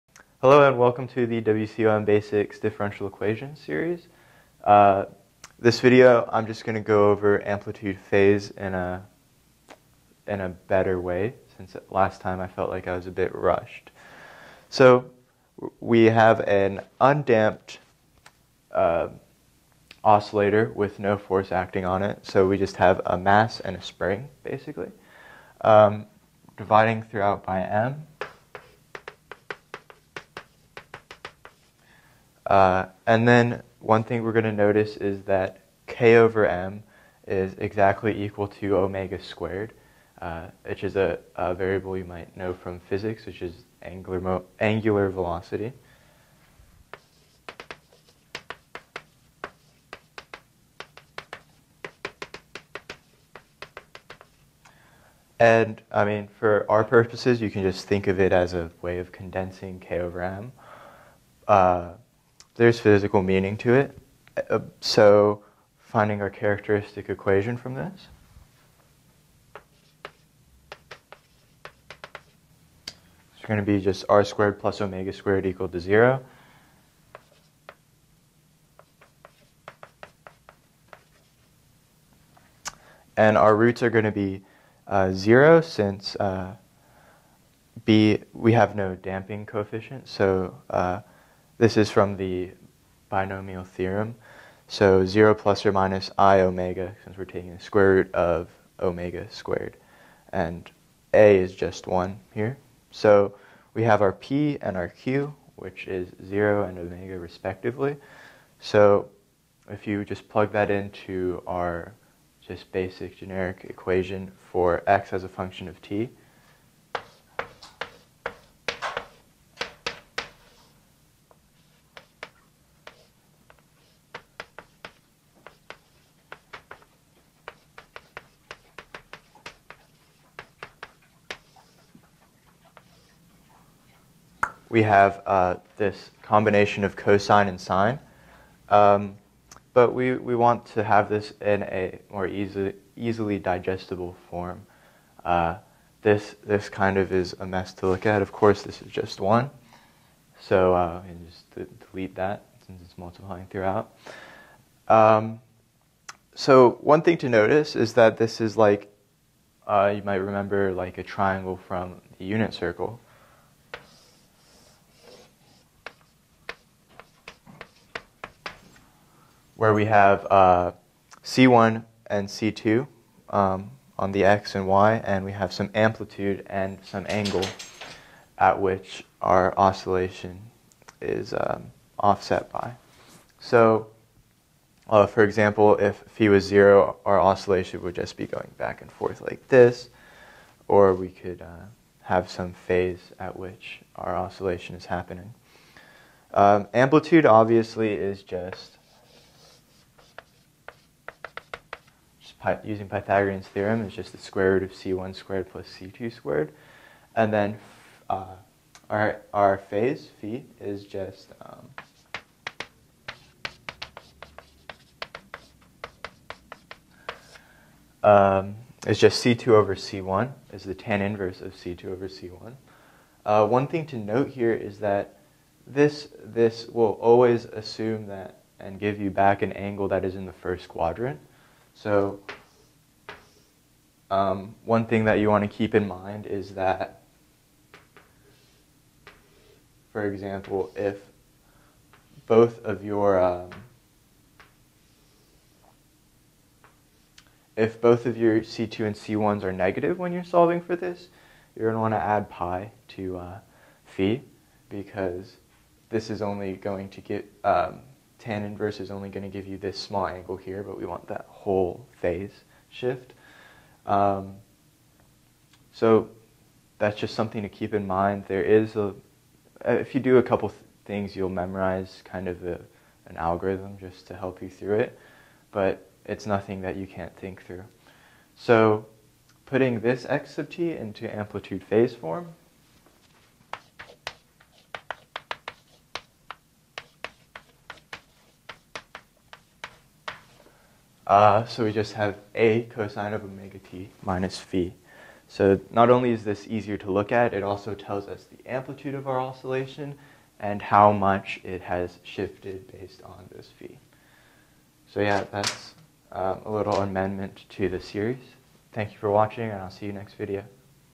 Amendment: Amplitude Phase Form
Re-touching on how to go from a general solution to amp-phase form.